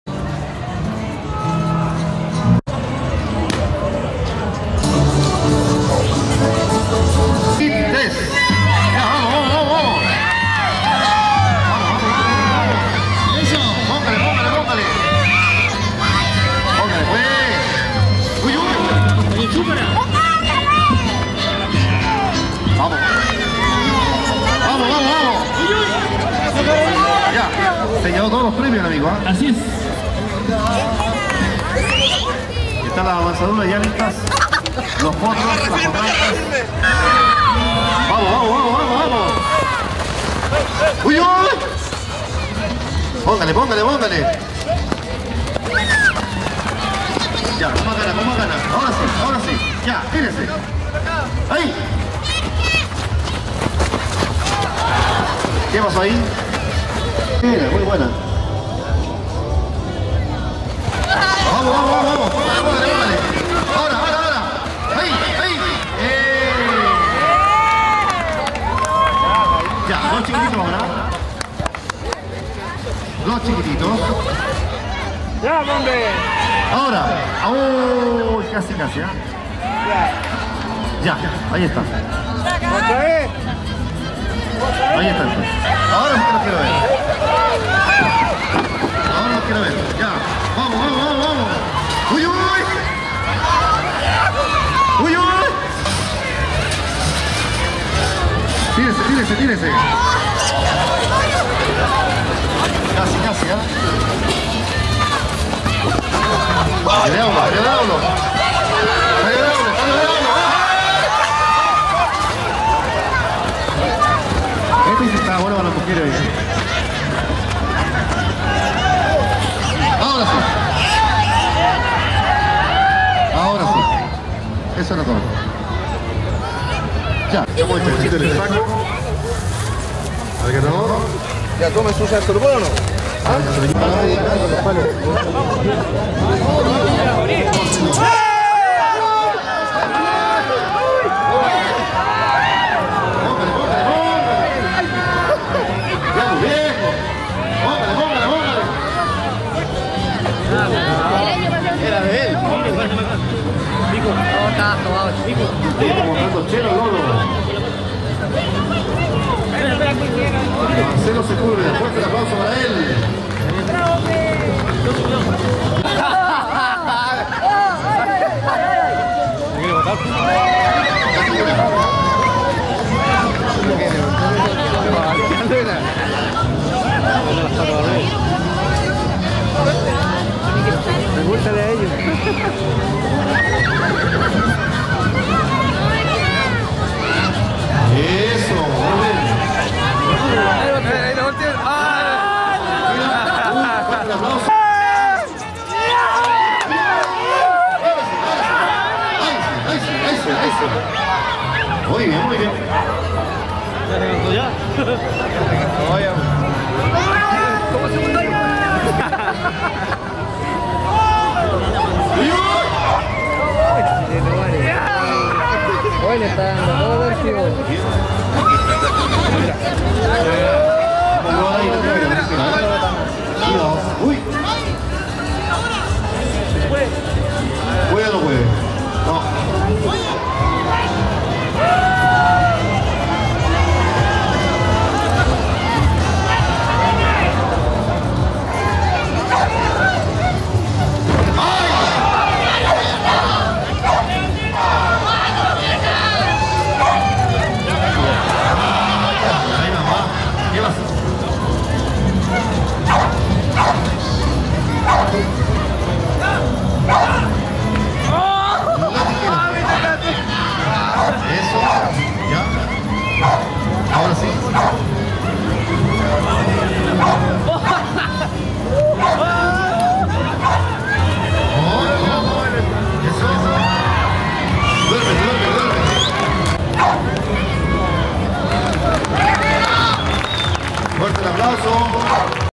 MEDIALUNA UNOmovie
Actividades realizadas en medialuna de San Carlos en celebración 18 de septiembre 2015